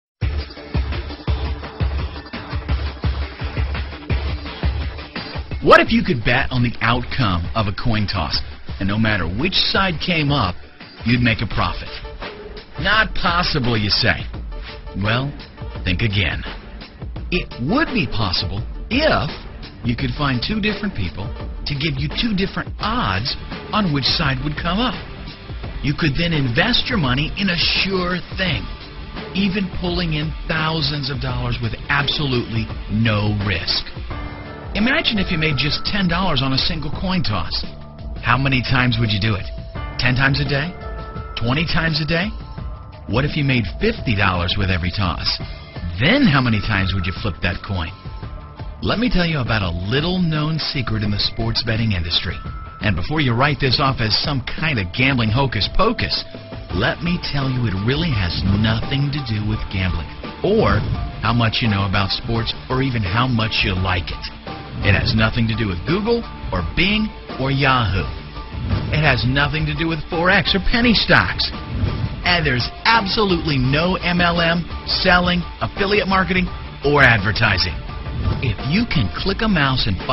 online sport gambling betting offers
Betting your whole life on one roulette spin
Crazy gamble with entire life savings one spin!.*2011 update* I've  learned of a way to win every time. Wow it is great :)
New up date we now have a IPhone APP!